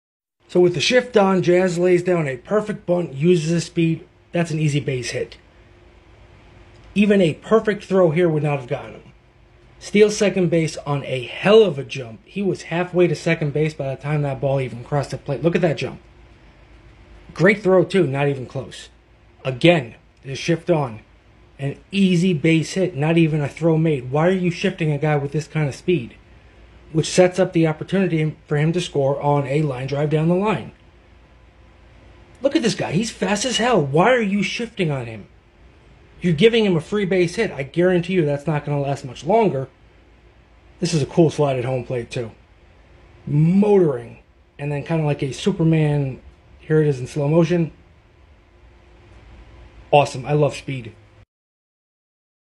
Jazz Chisholm Showing Off The Wheels ⚾️🔥
Jazz Chisholm Perfect bunts and speed

Grunttalksmlb.com